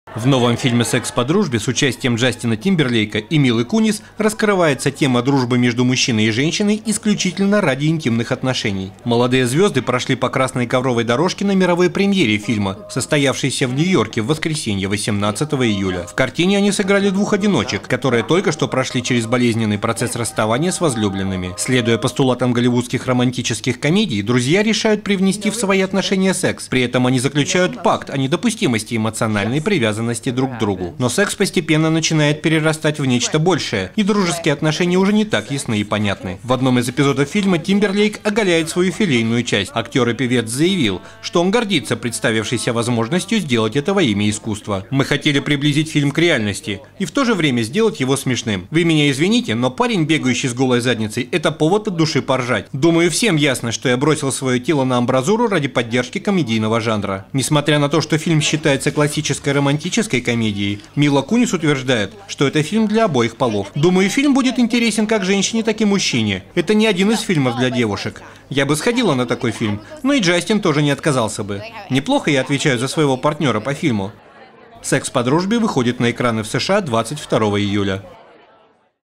Тимберлейк оголился ради искусства
Джастин Тимберлейк и Мила Кунис посетили премьеру своего фильма «Секс по дружбе».
Дата публикации: 20 июля 2011 18:09